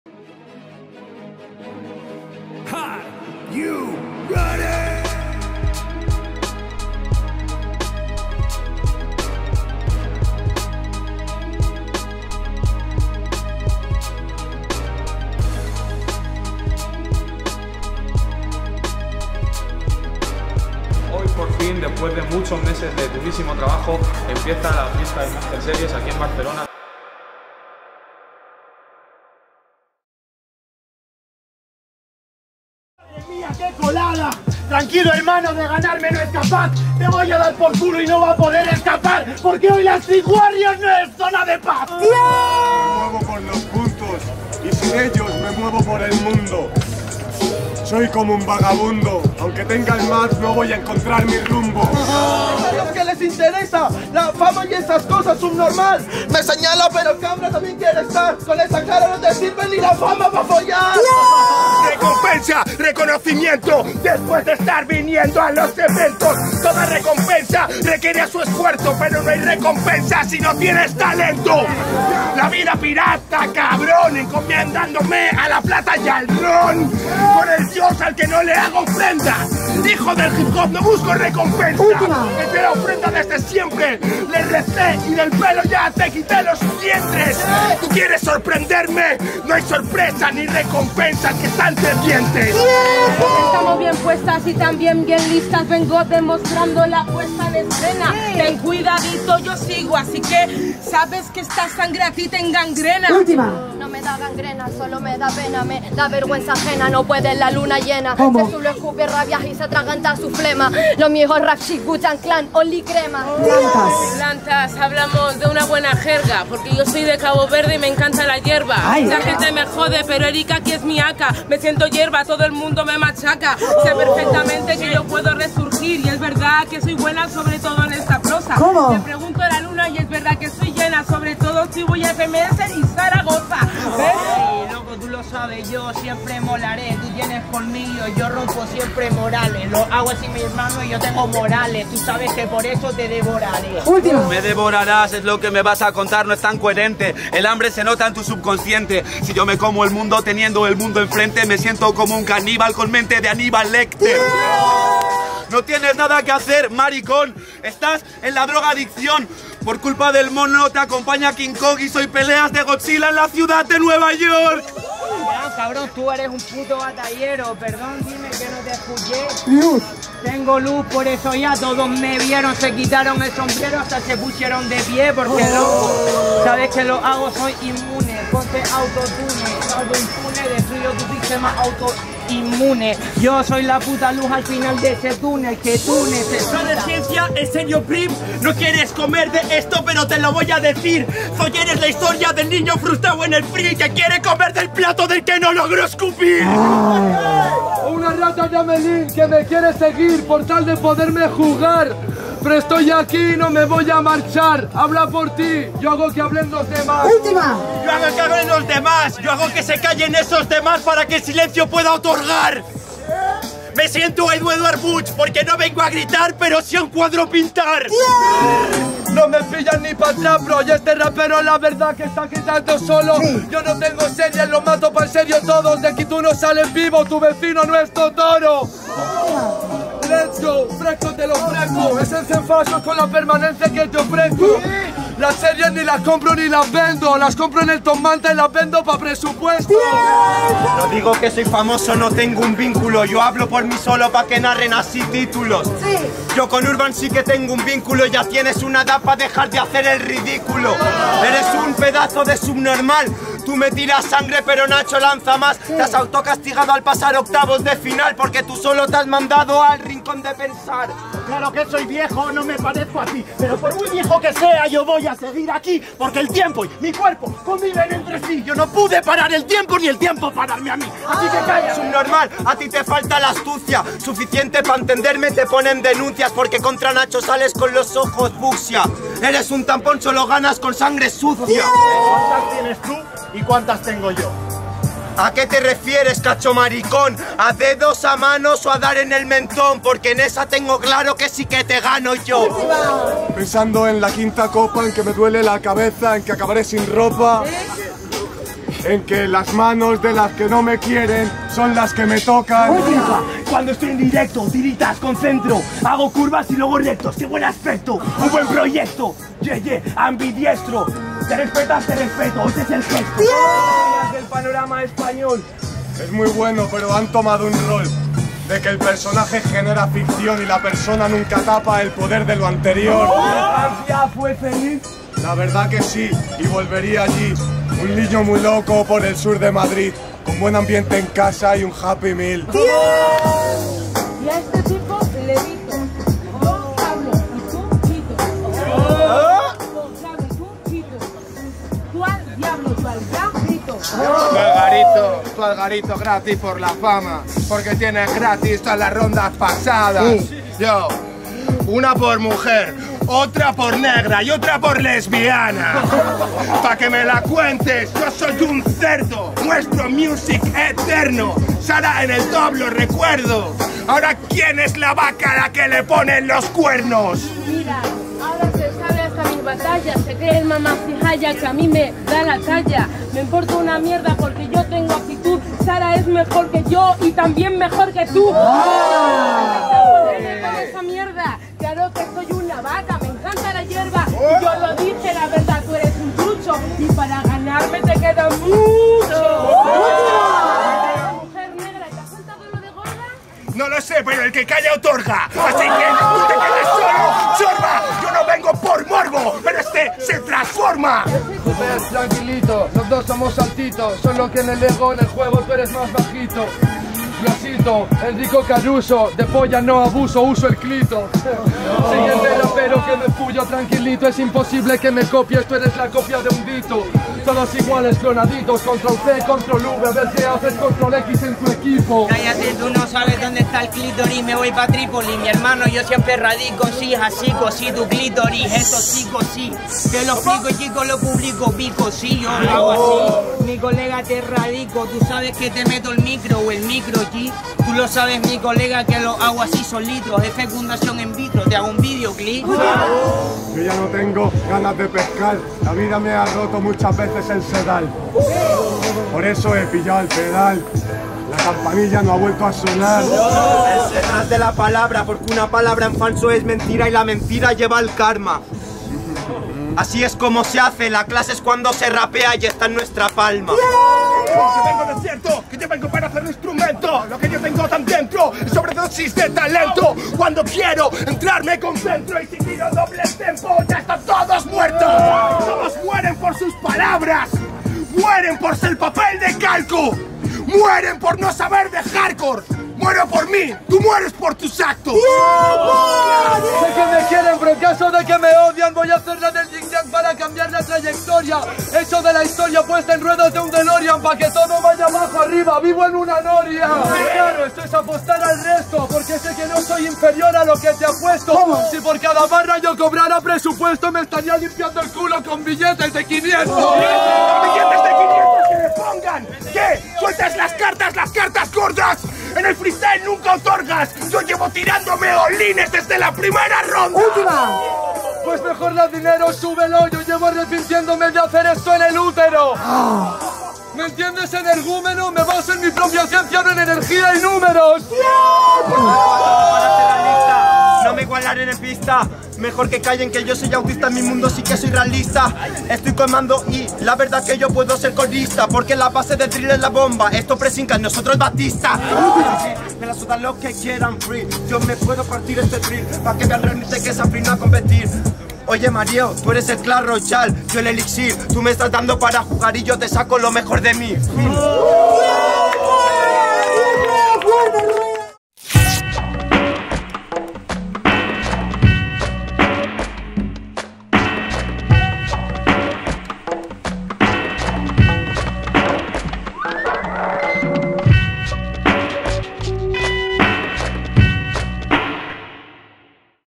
MEJORES RIMAS DE STREET WARRIORS 2022 - Jornada 5 | Urban Roosters
Street Warriors es la competencia del underground por excelencia, organizada por FRF y Urban Roosters. Su iniciativa es realizarla un día antes de cada jornada FMS para conocer el talento de la ciudad en donde se alberga la jornada. El ganador de la competencia clasifica a la gran Final Nacional que se posiciona como rango 2 del ascenso a FMS España y otorga 20.000 puntos al campeón.